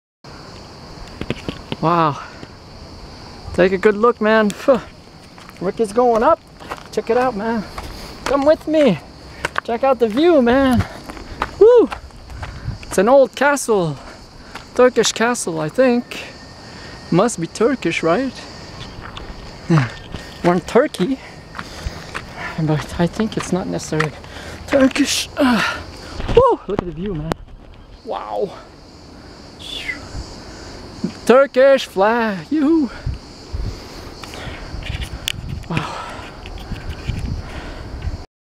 Watch Ricky climb to the top of Fethiye Castle !
I just happened to stumble on this ancient castle again by accident while walking on the road to the beach.  You don't see this castle unless you are walking since it is hidden from view but get a great view from the top !